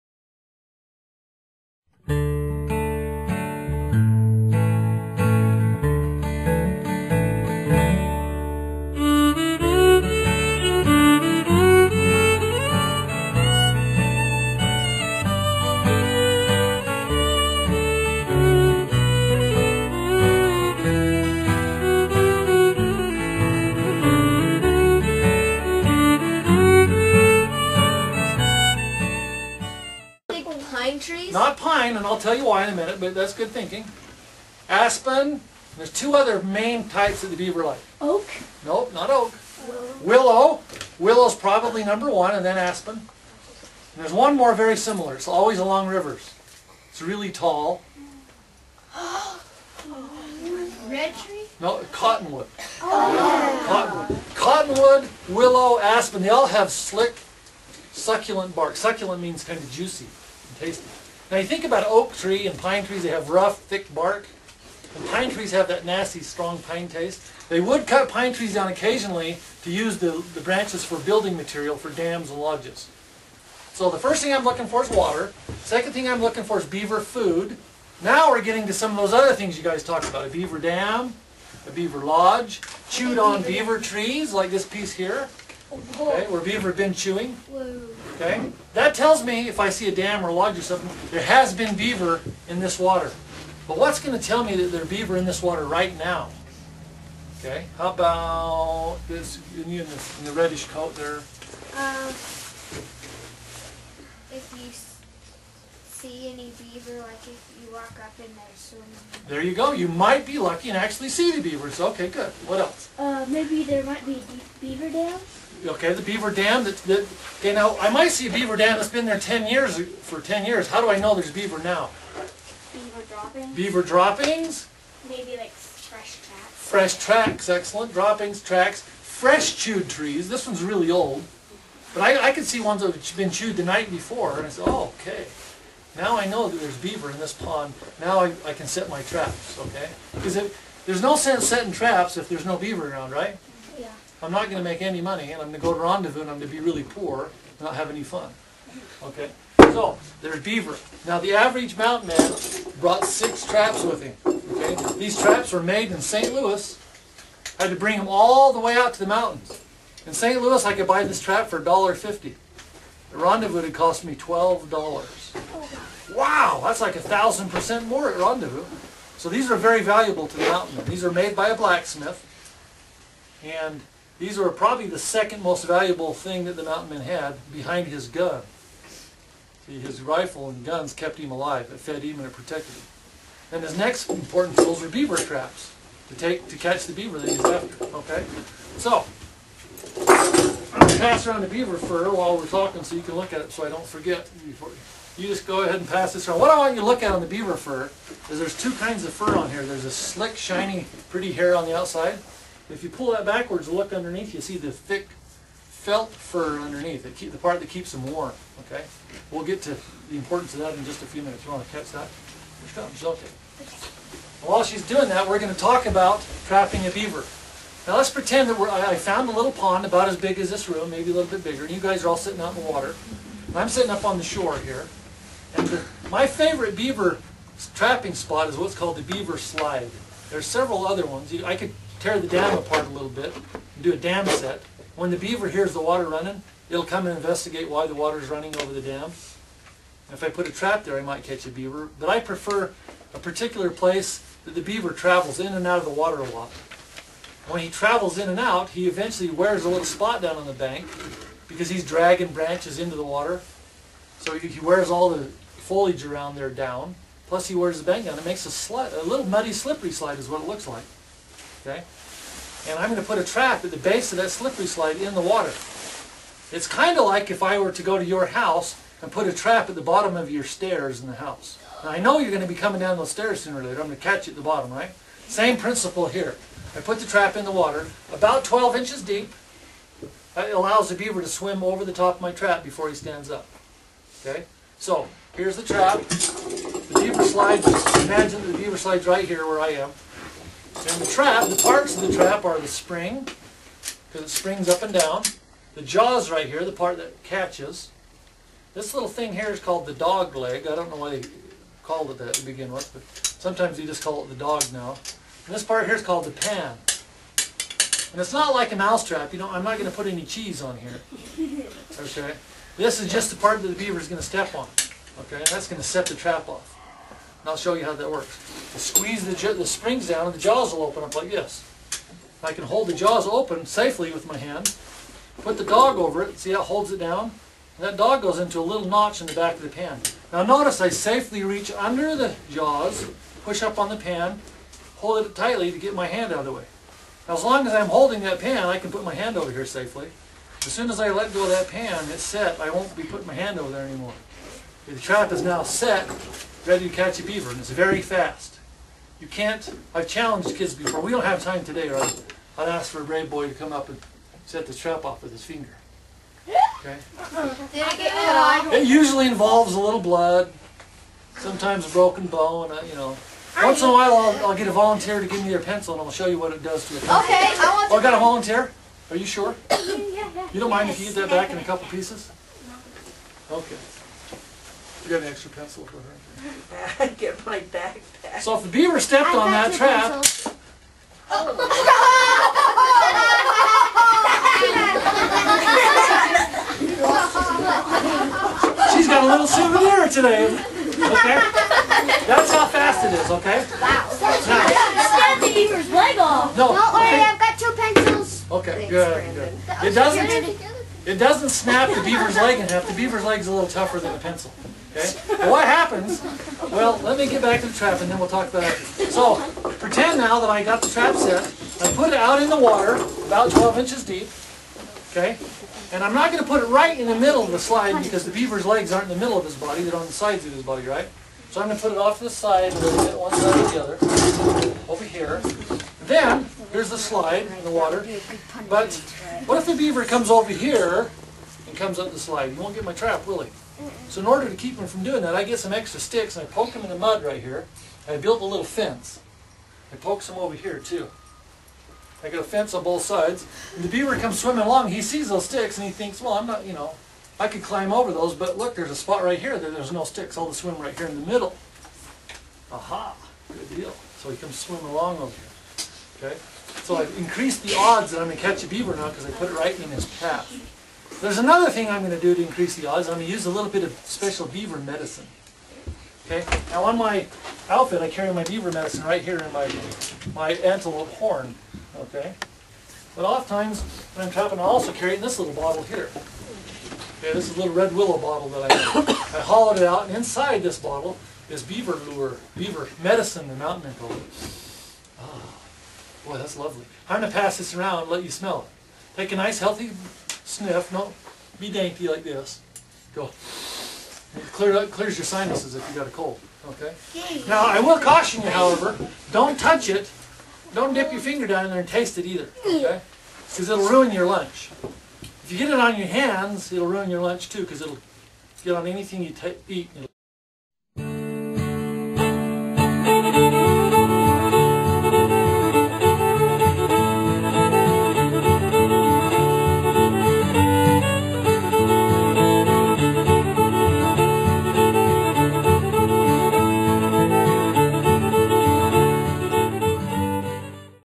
Historical Beaver Trapping Lecture- Part 2
A continuation of Teton Todd's lecture to a Wyoming 4th grade class on the why's and wherefores of beaver trapping by the original Mountaineers.